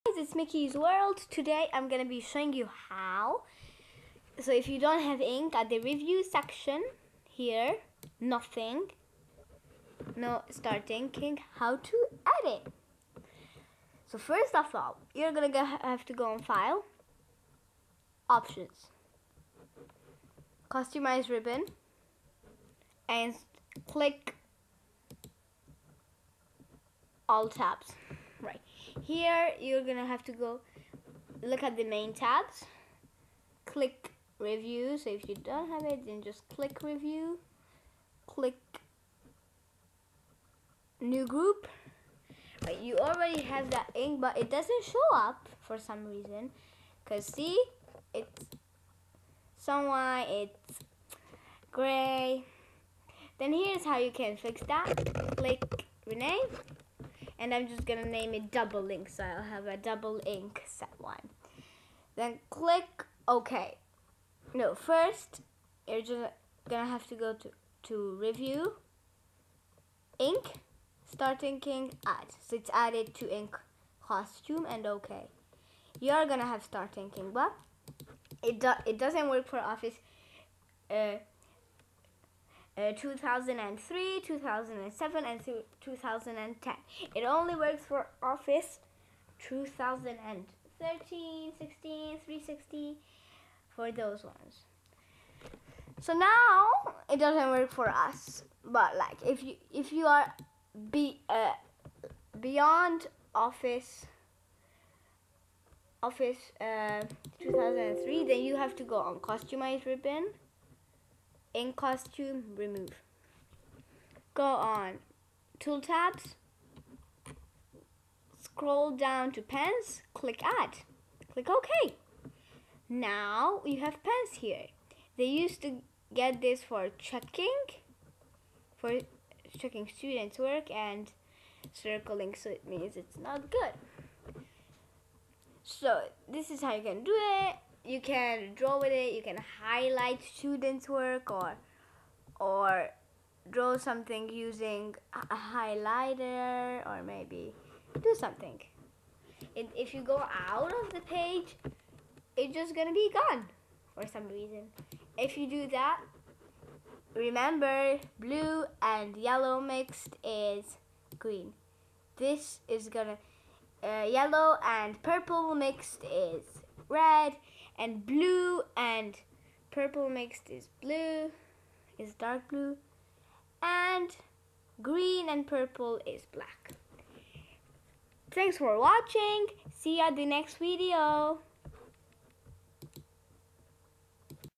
How to add start inking in Microsoft Word
Why don't I have "start inking" in Microsoft Word,Powerpoint, Excel and how to add it or if it doesn't work then how to add pens